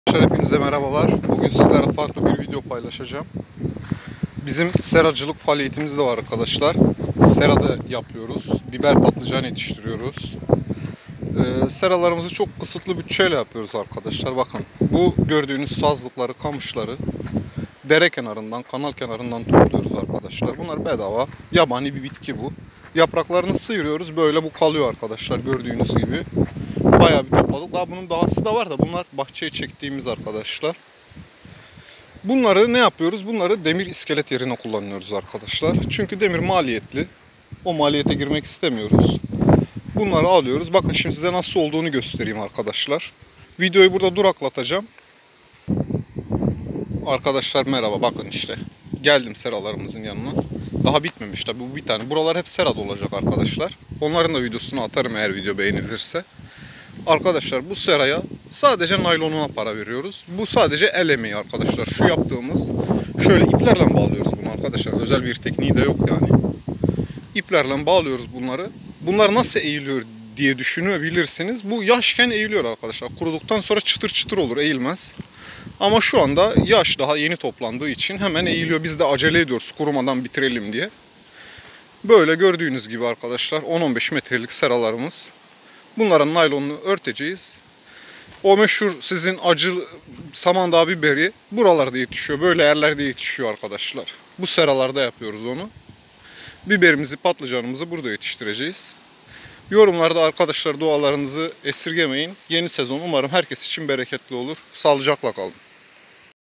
Düşük maliyetli sera yapimi | Meşhur Samandağ biberi nerede yetişiyor nasıl yetişiyor?
merhaba arkadaslar bugun hayvancilik videolarindan farkli bir video ile beraberiz size arazimizde kurdugumuz seralari maliyetimizi nasıl düşürdüğümüzü hangi teknikleri kullandigimizdan bahsedeceğim